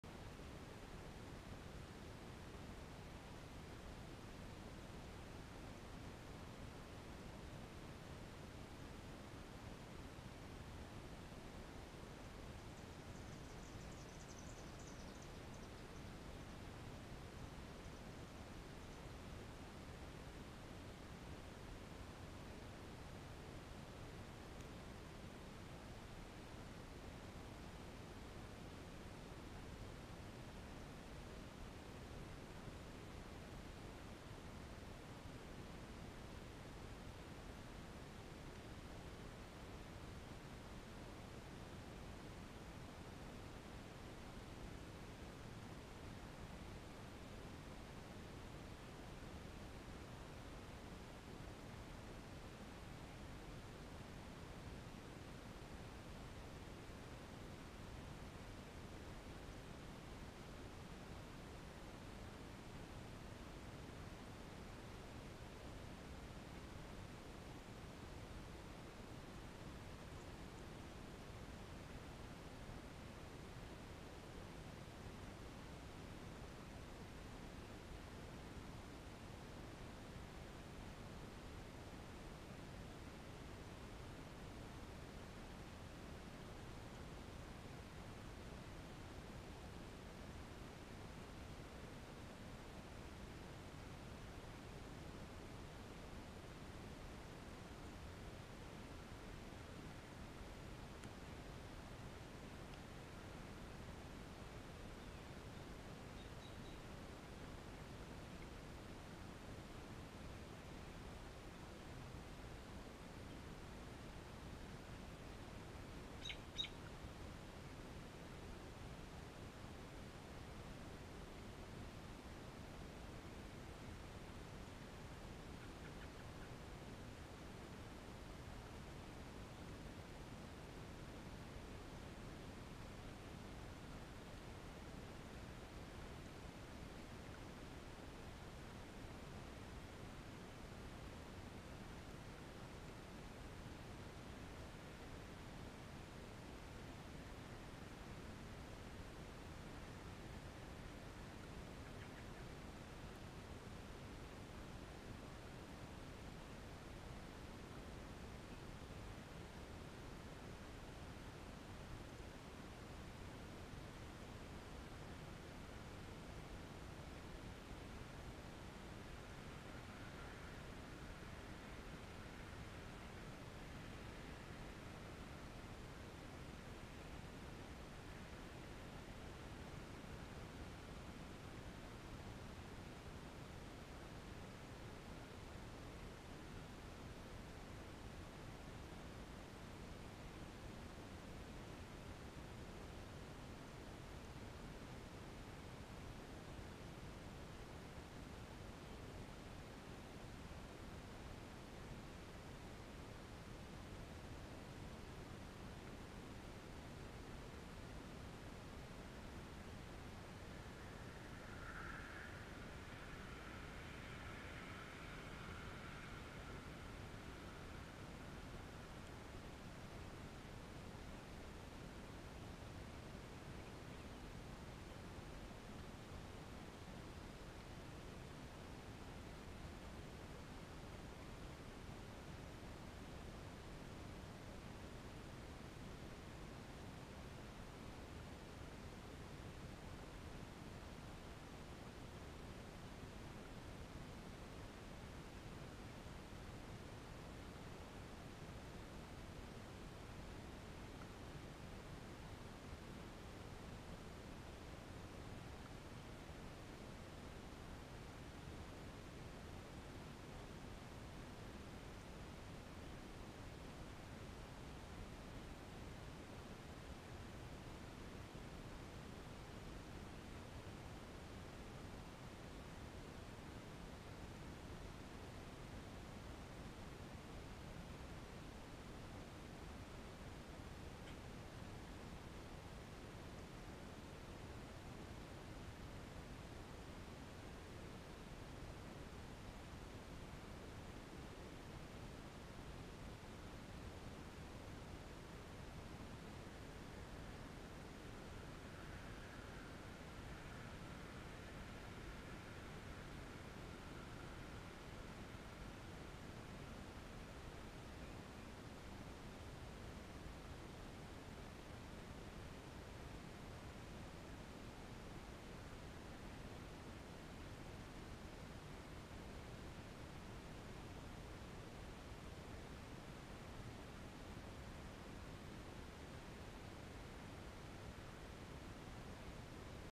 Mother and Baby Groundhog Eating First Watermelon
Checkout this awesome upclose footage of a family of woodchuck / groundhogs eating and loving watermelon.  This is the first time that they have ever experienced watermelon, and they loved it!